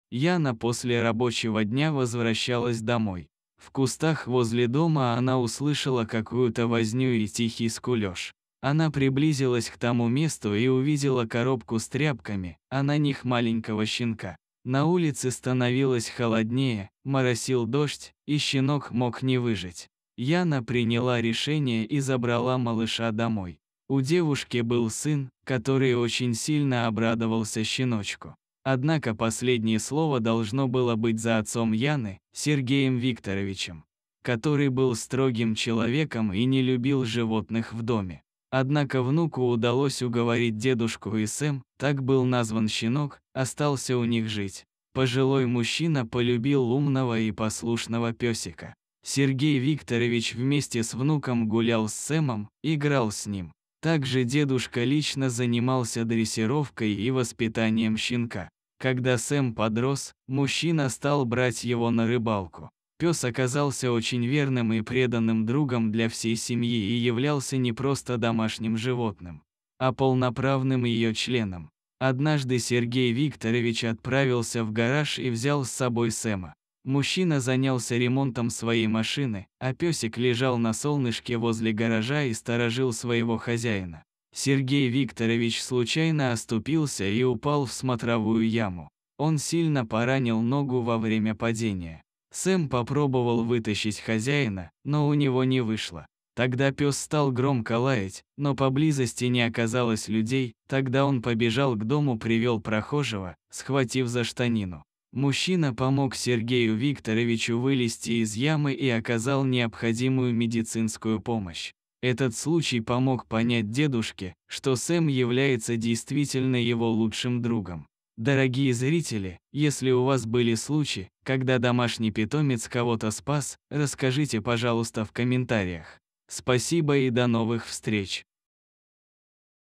Девушка подобрала бездомного щенка, а позже ее ждал сюрприз!
Яна после рабочего дня возвращалась домой. В кустах возле дома она услышала какую-то возню и тихий скулеж. Она приблизилась к тому месту и увидела коробку с тряпками, а на них маленького щенка. На улице становилось холоднее, моросил дождь, и щенок мог не выжить. Яна приняла решение и забрала малыша домой. У девушки был сын, который очень сильно обрадовался щеночку.